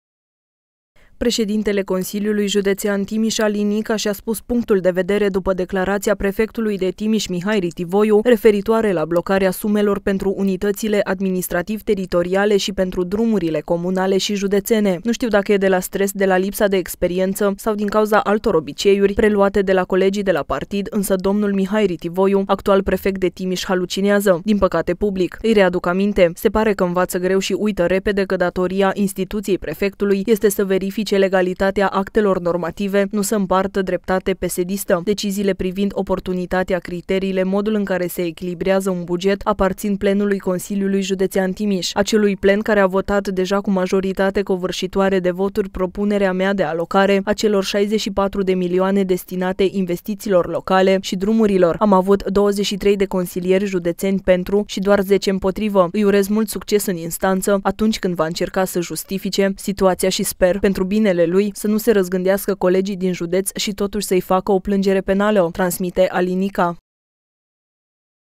Alin Nica: „Mihai Ritivoiu halucinează!”
Președintele Consiliului Județean Timiș, Alin Nica, și-a spus punctul de vedere după declarația prefectului de Timiș, Mihai Ritivoiu, referitoare la blocarea sumelor pentru unitățile administrativ-teritoriale și pentru drumurile comunale și județene. "Nu știu dacă e de la stres, de la lipsa de experiență sau din cauza altor obiceiuri preluate de la colegii de la partid, însă domnul Mihai Ritivoiu, actual prefect de Timiș, halucinează. Din păcate, public. Îi readuc aminte - se pare că învață greu și uită repede - că datoria Instituției Prefectului este să verifice legalitatea actelor normative, nu să împartă dreptate PSD-istă. Deciziile privind oportunitatea, criteriile, modul în care se echilibrează un buget, aparțin plenului Consiliului Județean Timiș